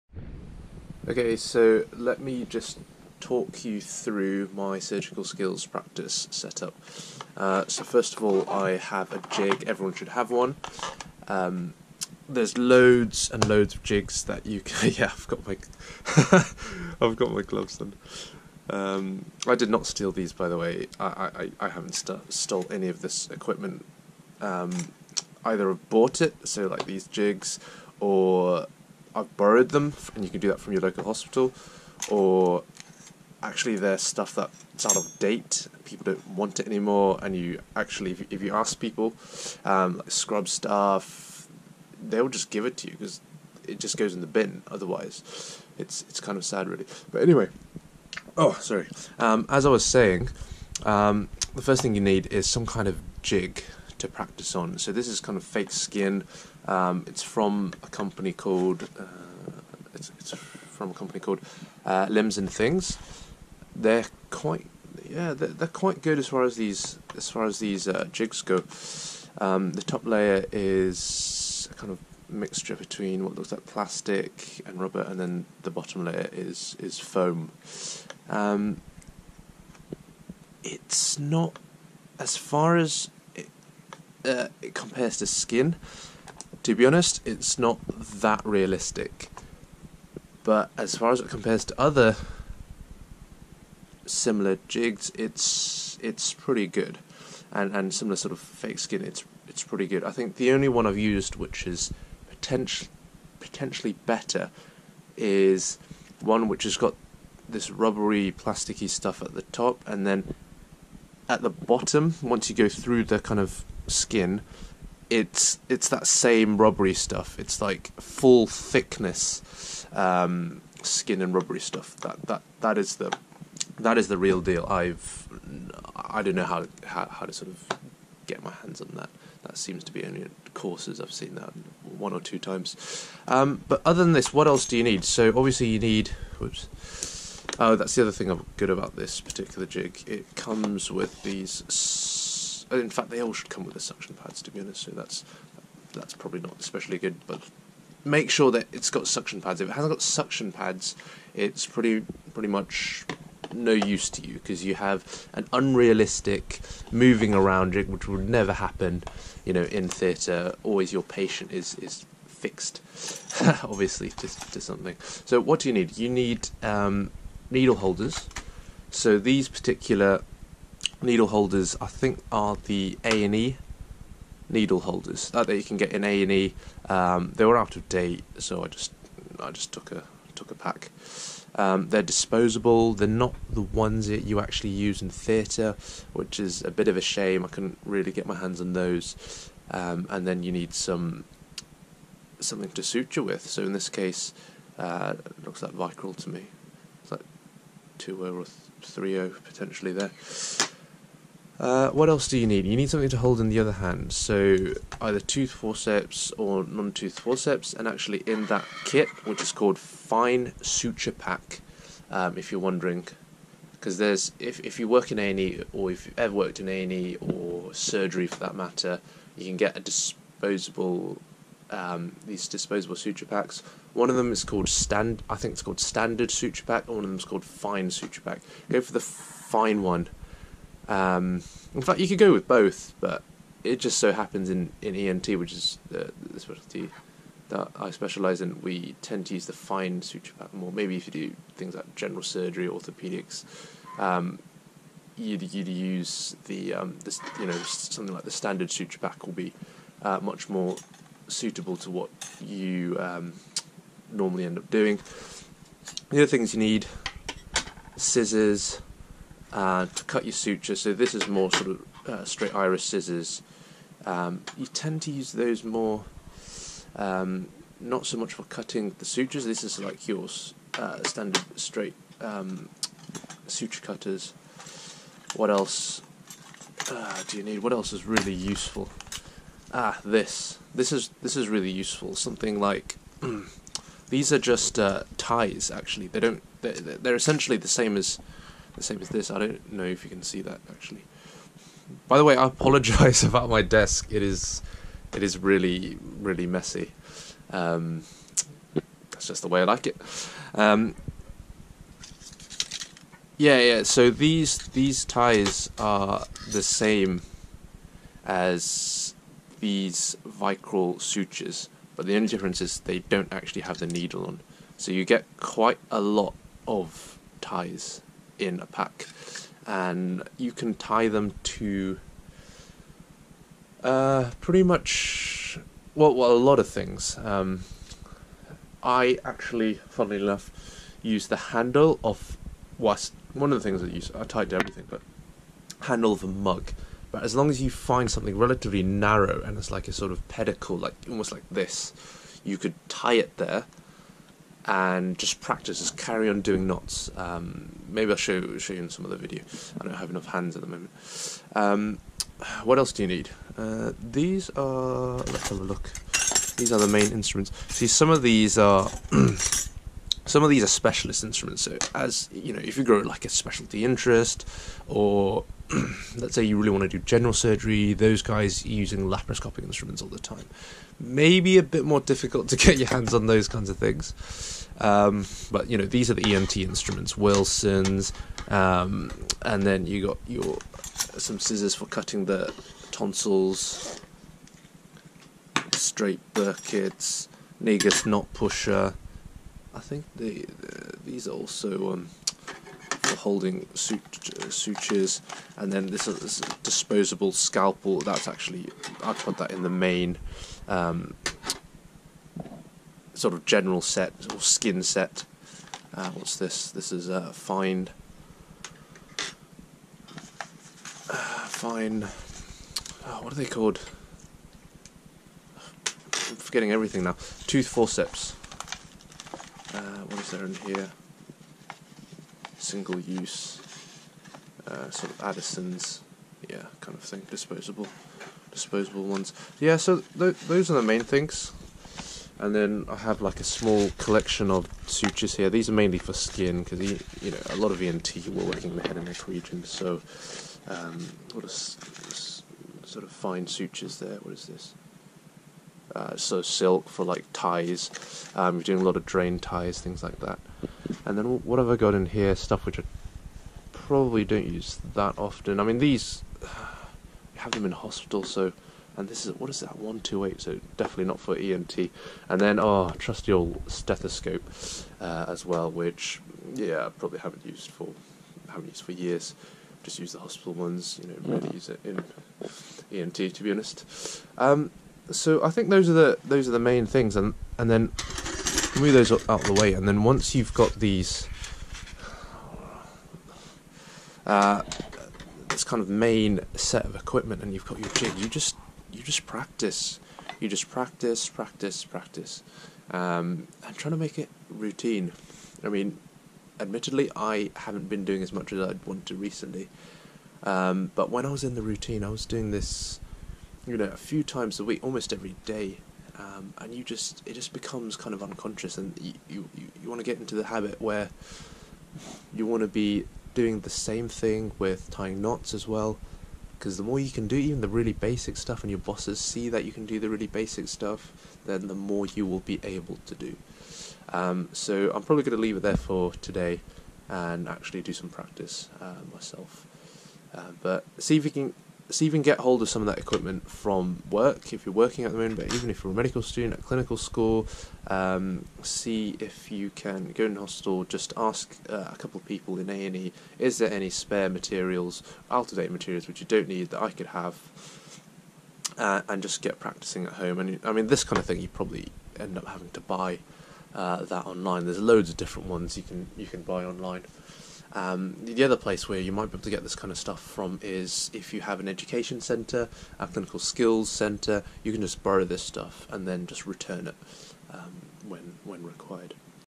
Setting up your Surgical Jig
I describe my set up for a basic surgical skin suturing jig using equipment which can be readily borrowed from hospitals, clinical skills departments, etc.